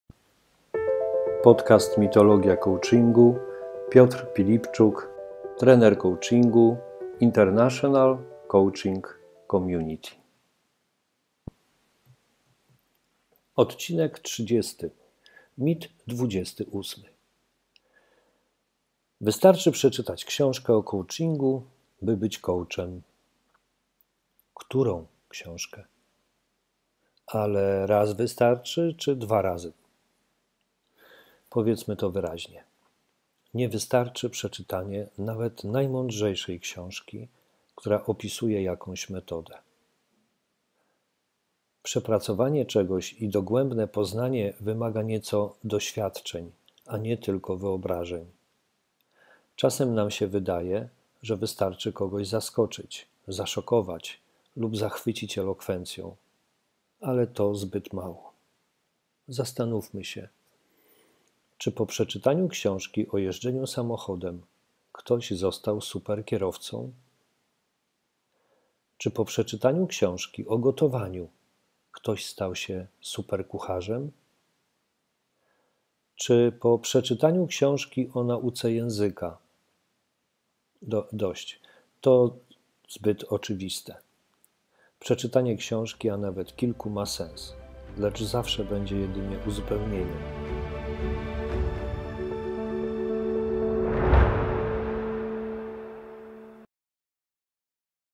Podcast - Mitologia coachingu - #30 "Mit 28 - wystarczy przeczytać książkę"
Podcast jest odświeżoną oraz rozszerzoną wersją książki "Współczesna mitologia coachingu". W każdym odcinku będziemy poruszać tematy dotyczące najczęściej zadawanych pytań dotyczących coachingu.

Życzymy miłego słuchania.